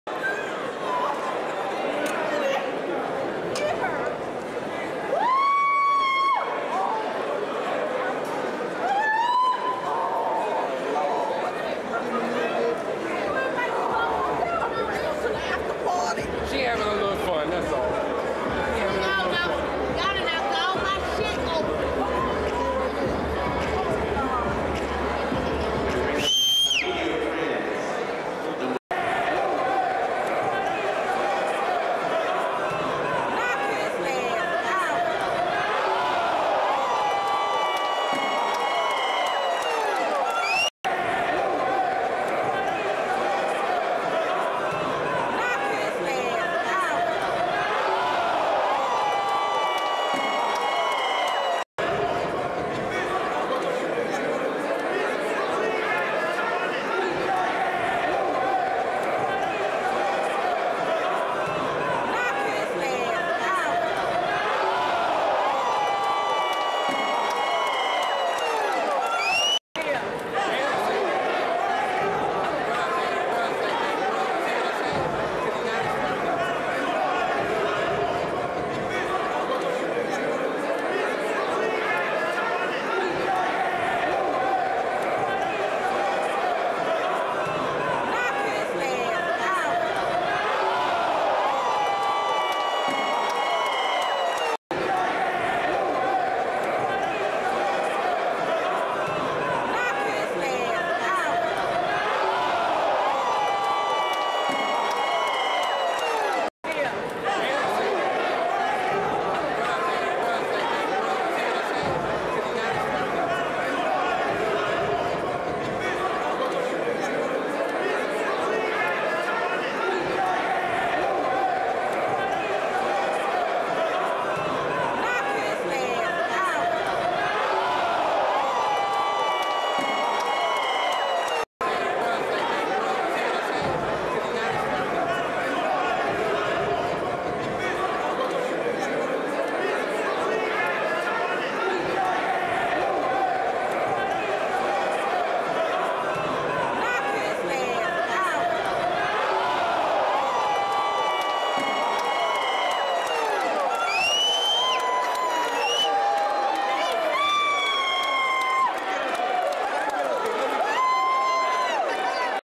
Henry Buchanan 3rd Round KO
Henry Sugar Poo Buchanan knocks out opponent in the 3rd round.